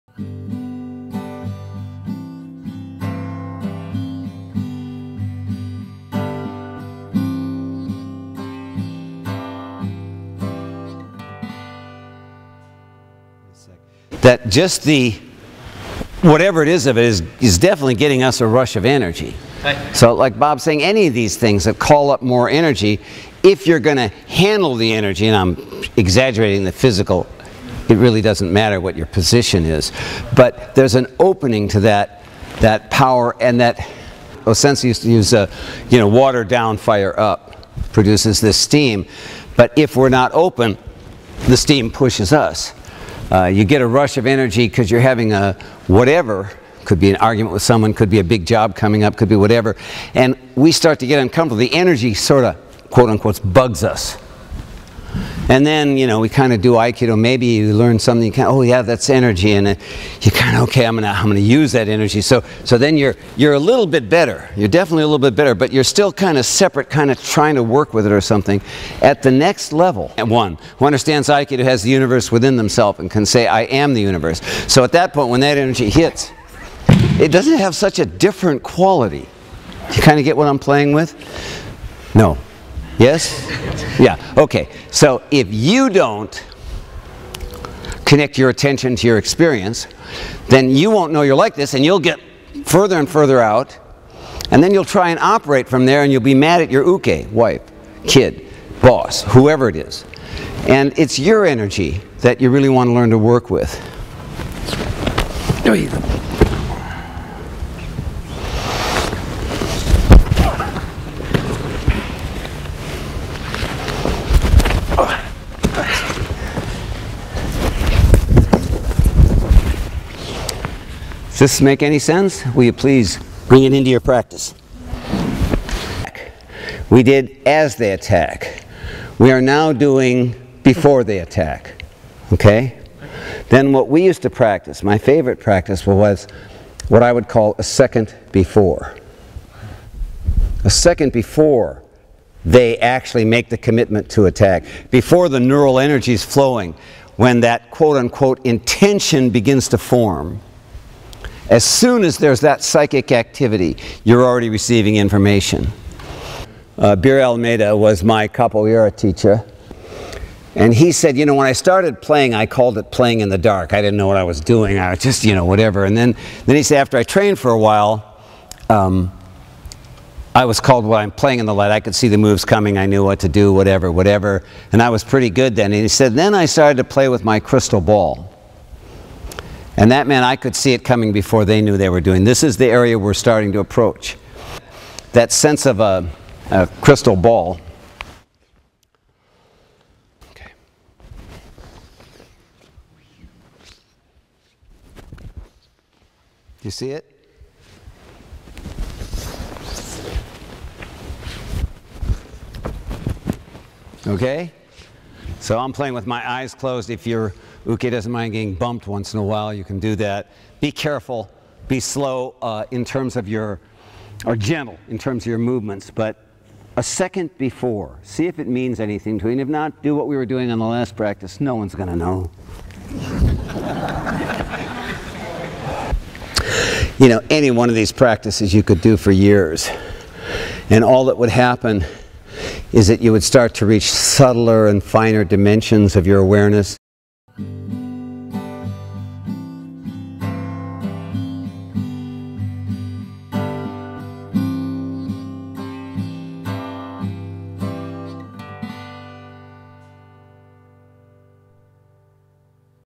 O S R Down Under 2019: Moonsensei Down Under IV
O S R Down Under 2019: Auckland, Steiner School, Sunday 17 March 2019